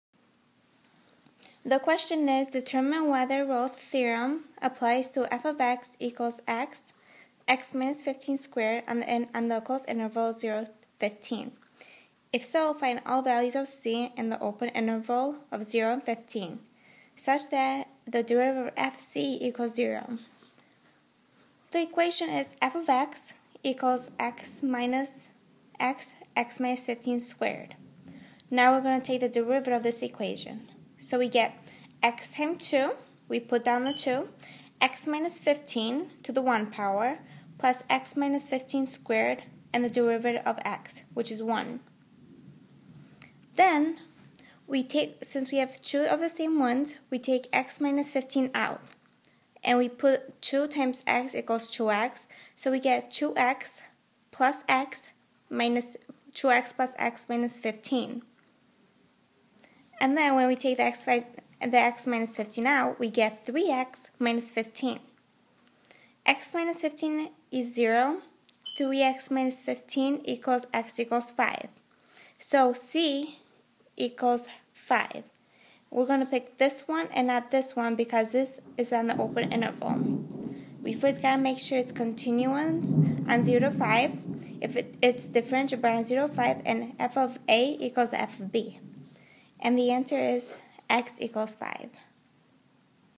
How to use Rolle's Theorem to determine the value(s) of c of f(x)=x(x-15)^2 in (0,15)
Determine whether Rolle's Theorem applies to f(x)=x(x-15)^2  on [0,15]. If so, find all values of c in the open interval (0,15) such that  f'(c)=0.